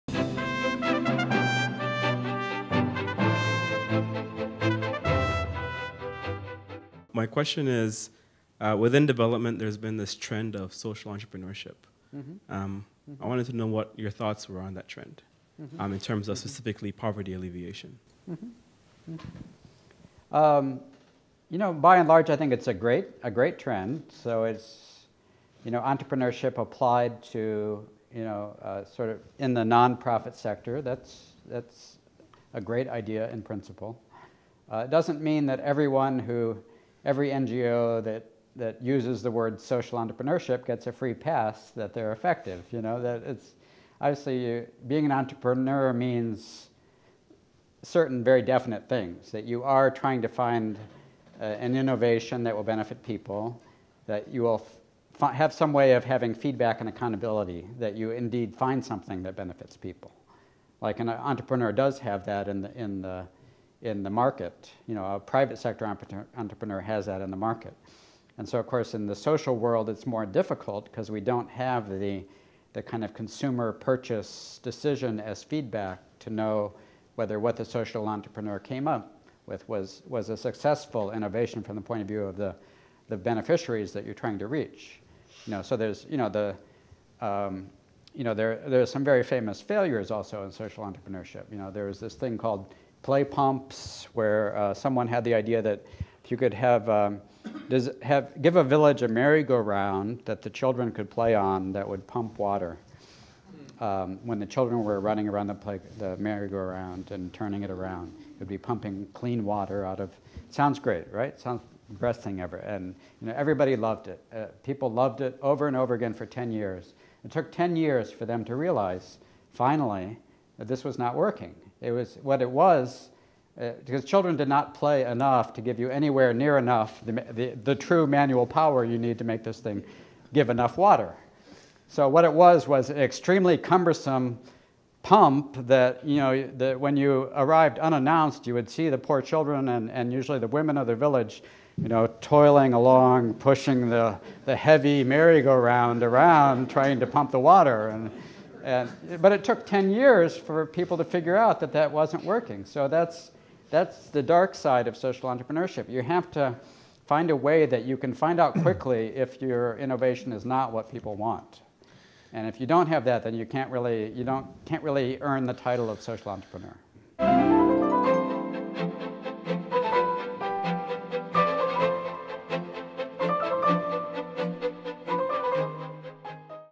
William Easterly: Social Entrepreneurship
William Easterly discusses the positives and negatives of social entrepreneurship.

This Carnegie Council event took place on Thursday, September 15, 2011. For complete video, audio, and transcript, go